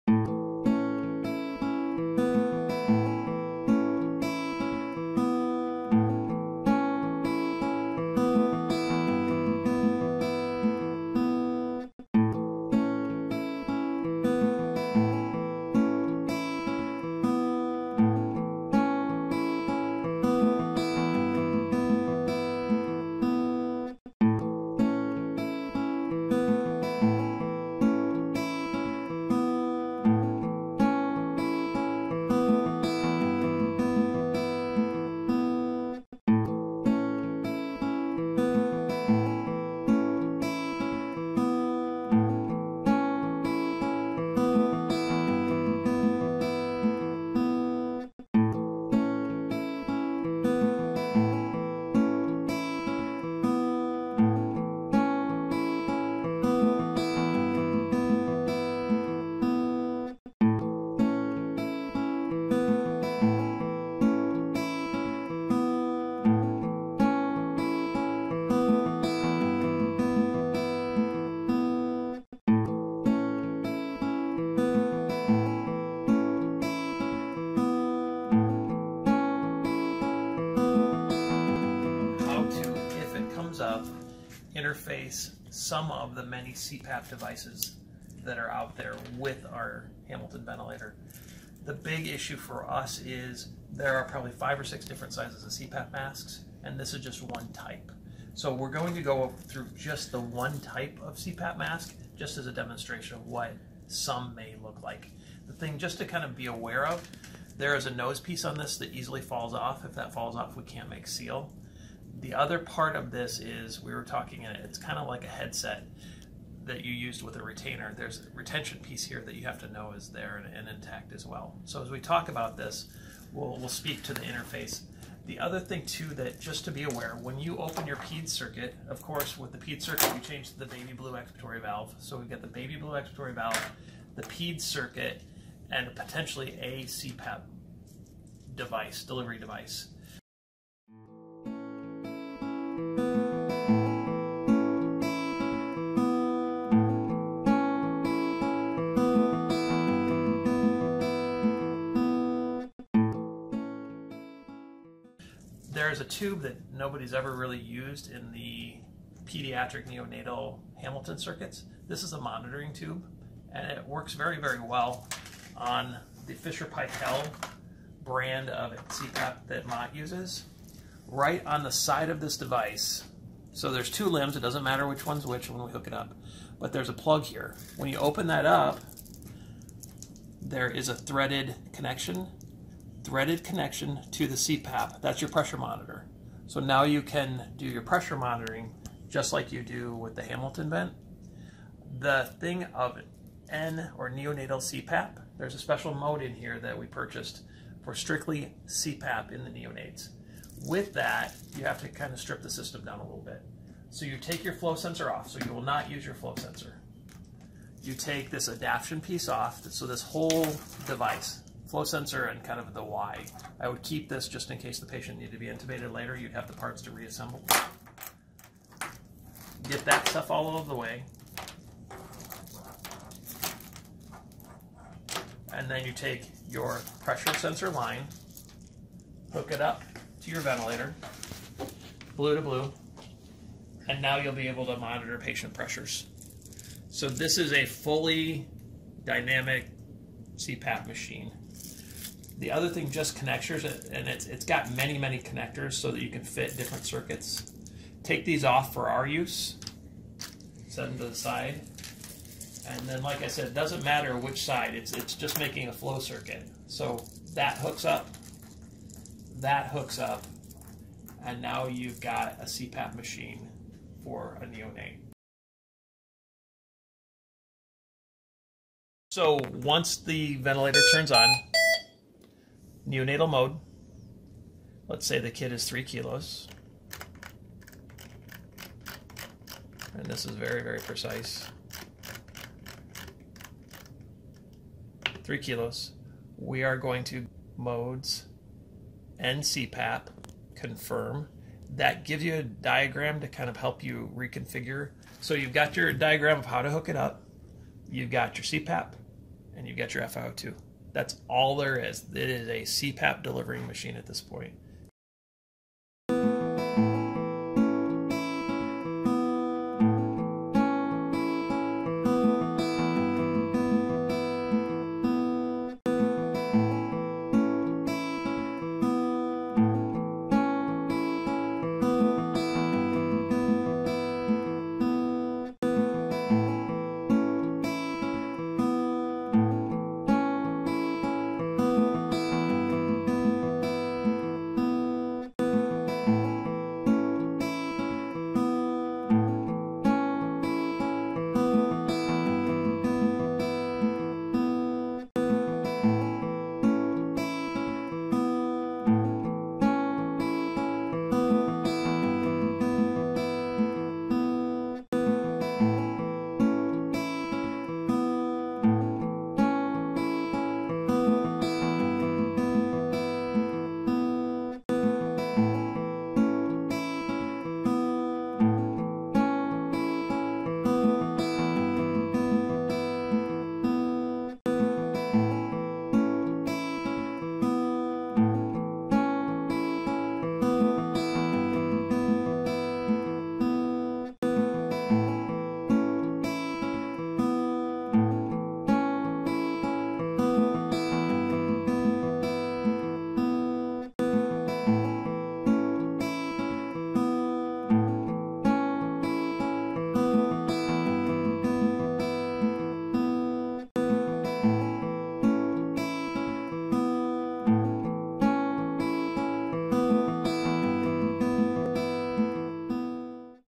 Neonatal CPAP on the Hamilton T1
One possible solution to the clinical challenge of providing non-invasive ventilation to our smallest patients